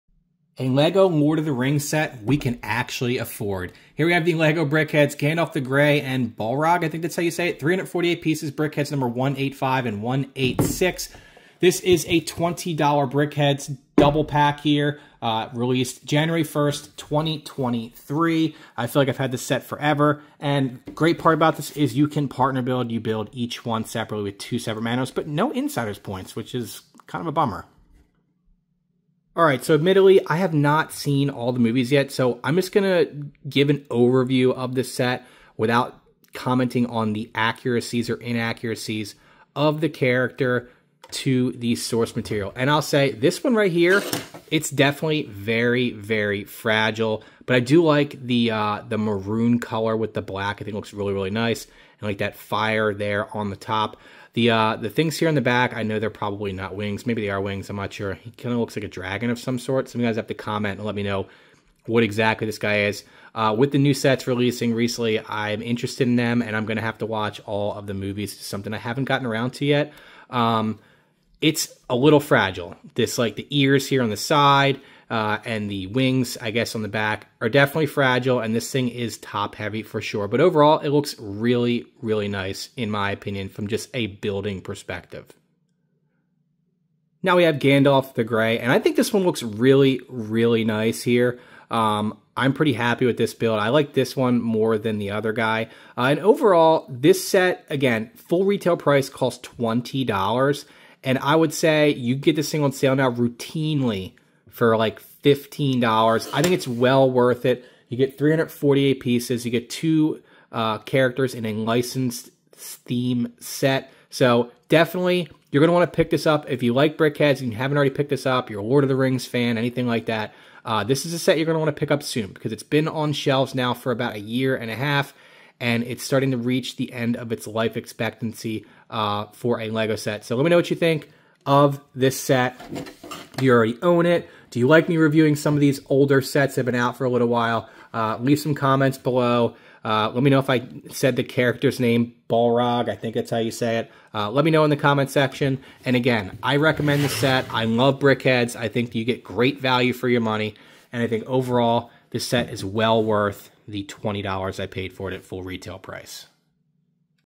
Lord of the Rings Set Fans CAN AFFORD! Brickheadz 40631 Review!
Review of the Lego Lord of the Rings Brickheadz Gandalf the Grey and Balrog set 40631. With the very expensive Lord of the Rings Lego sets released in the last year, I knew it was time to build and review this set!
I am a huge fan of Lego Brickheadz. The Lego Brickheadz LOTR set 40631 is priced at an affordable $20, released on January 1st, 2023, and its affordable! Lego Rivendell is not affordable.

What do you think of this set?

Lego 40631 review. Lego Brickheadz 40631. Lego Gandalf. Lego Balrog. Lego LOTR. Lego Lord of the Rings.